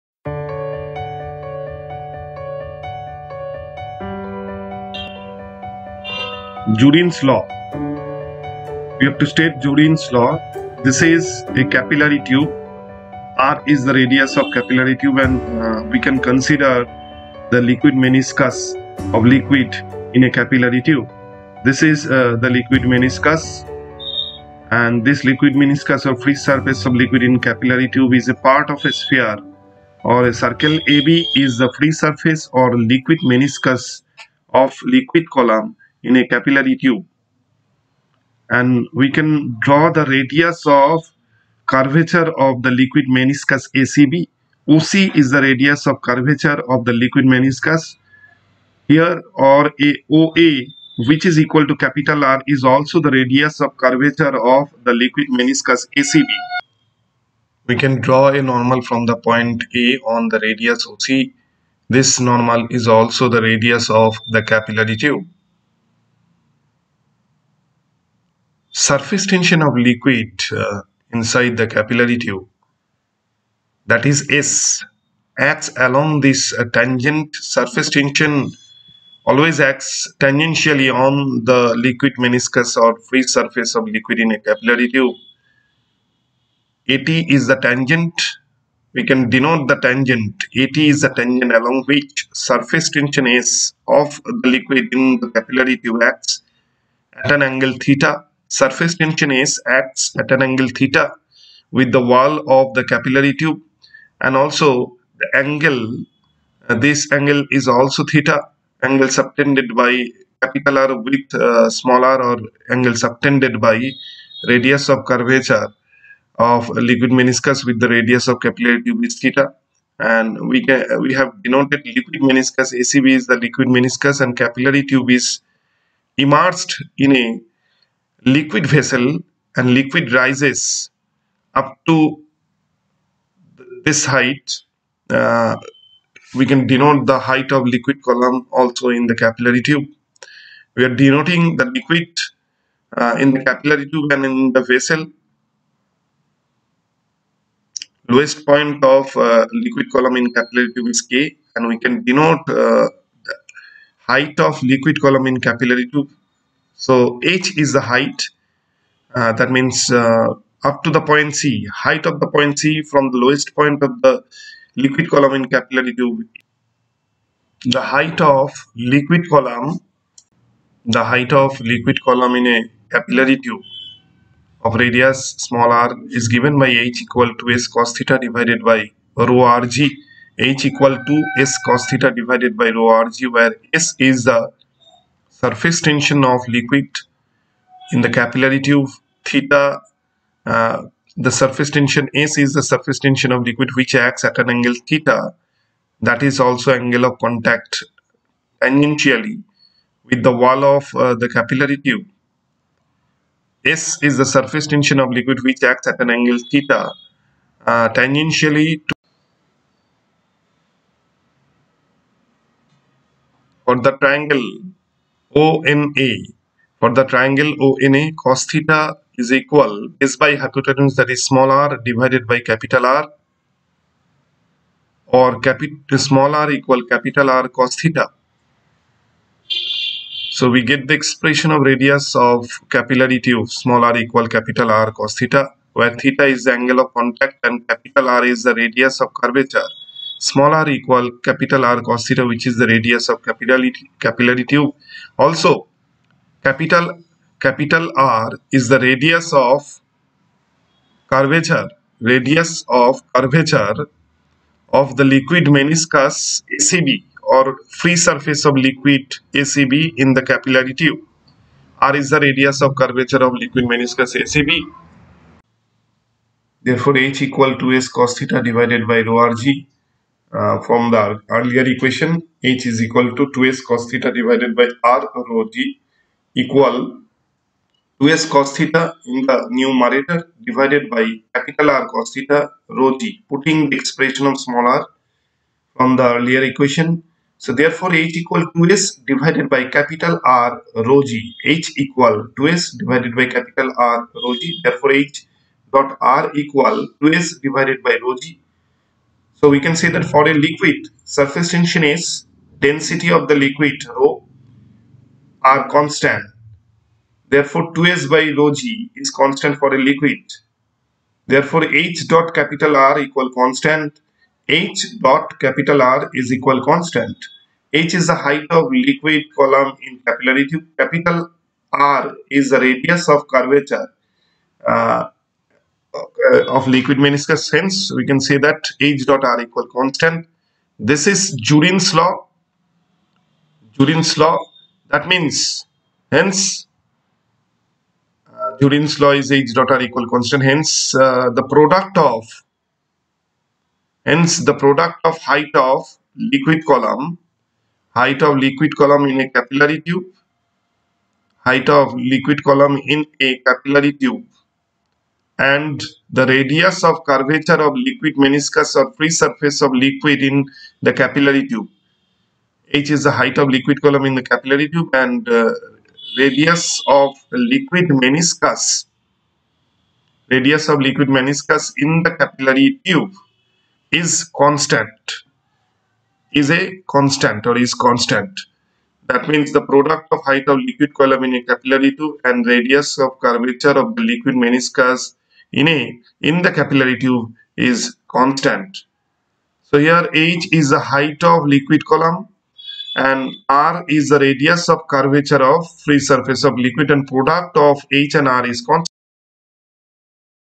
Jurin's Law // Surface Tension
Chapter: Surface Tension : State Jurin's law .

For a liquid in a capillary tube ,
h. R = constant This is Jurin's law...
where,
                   R = radius of curvature of liquid meniscus and
                    h = height of liquid column.

* If a capillary tube .of insufficient length is placed vertically in a liquid, then liquid never come out from the tube on its own.

* Jurin's law, or capillary rise, is the simplest analysis of capillary action—the induced motion of liquids in small channels—and states that the maximum height of a liquid in a capillary tube is inversely proportional to the tube's diameter. Capillary action is one of the most common fluid mechanical effects explored in the field of microfluidics. Jurin's law is named after James Jurin, who discovered it between 1718 and 1719. His quantitative law suggests that the maximum height of liquid in a capillary tube is inversely proportional to the tube's diameter. The difference in height between the surroundings of the tube and the inside, as well as the shape of the meniscus, are caused by capillary action. The mathematical expression of this law can be derived directly from hydrostatic principles and the Young–Laplace equation. Jurin's law allows the measurement of the surface tension of a liquid and can be used to derive the capillary length.